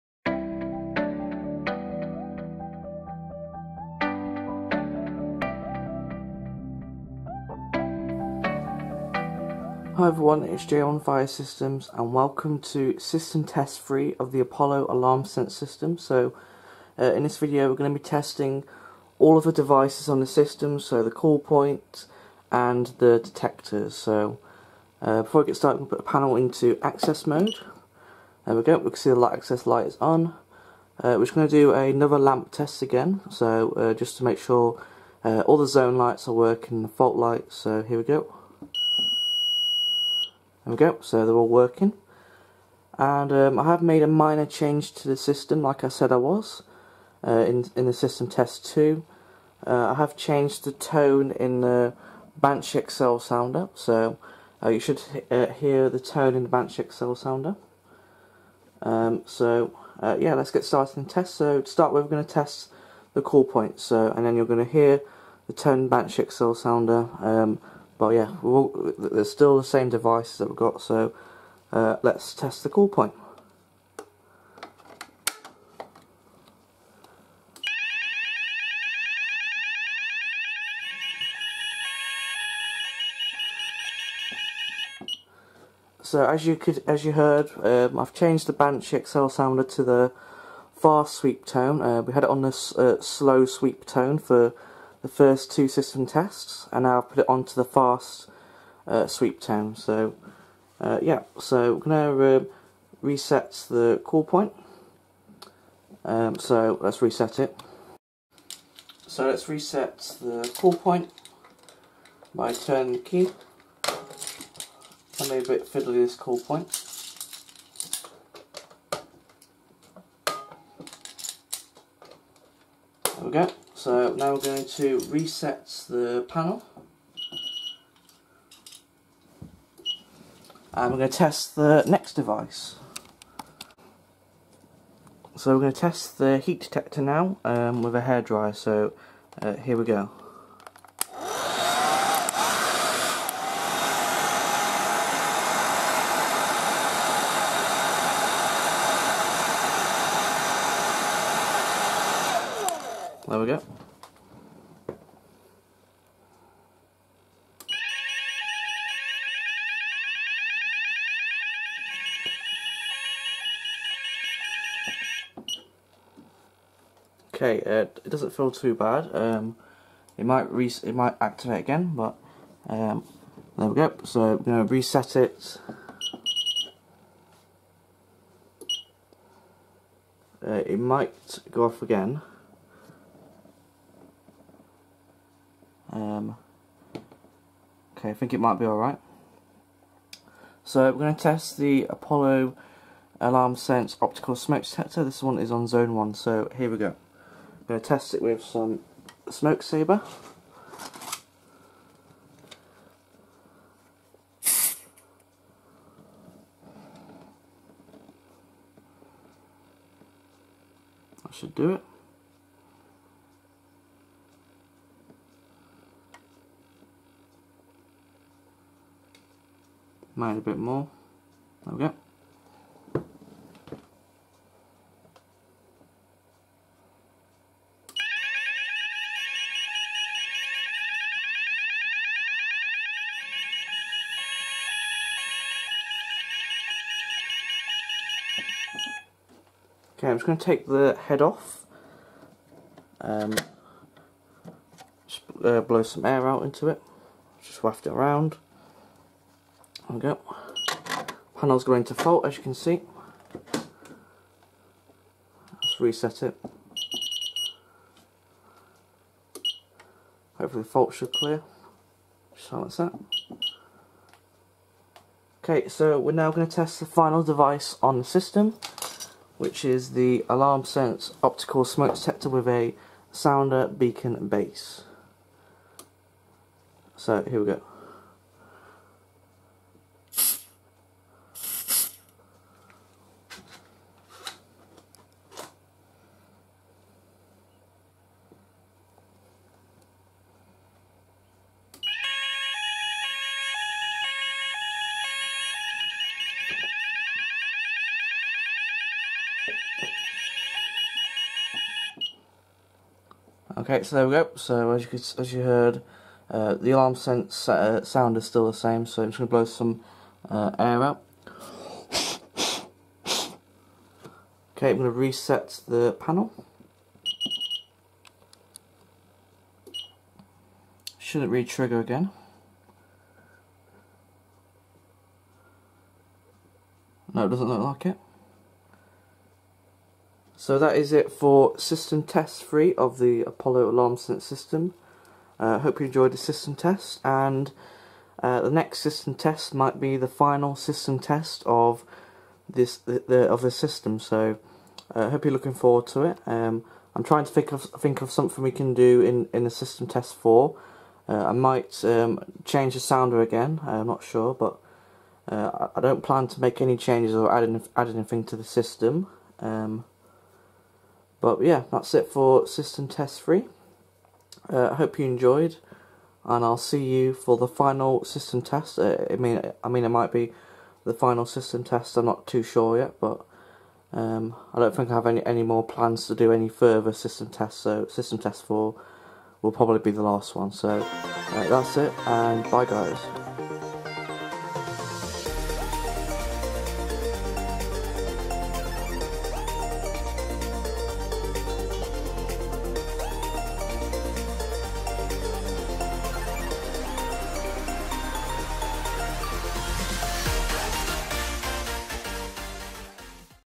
Apollo Alarmsense System Test 3
Welcome to system 3. Today I test all the detectors and call points in the system.